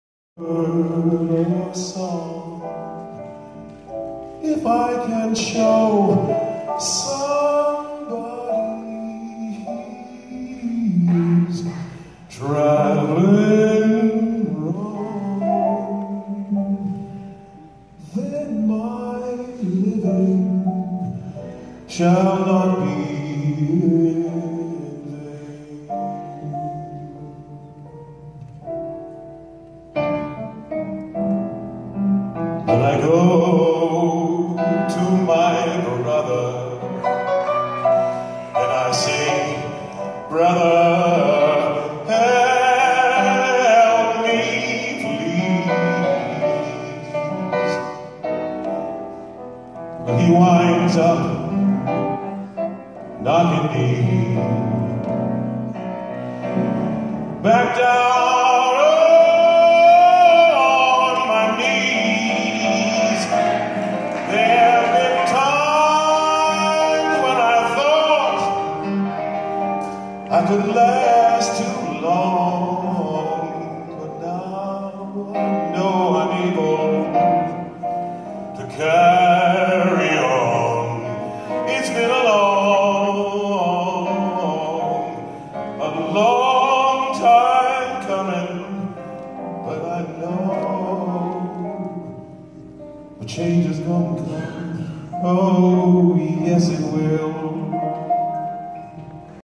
Rev Dr Joseph Lowery 96th birthday celebration
Actor Mr Keith David singing